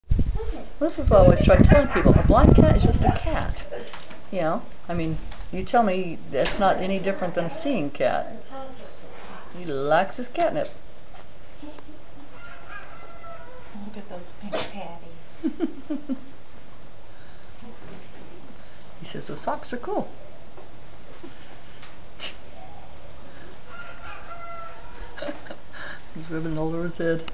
CASPER.MOV
Another Casper and a catnip toy.  He loves cat nip :)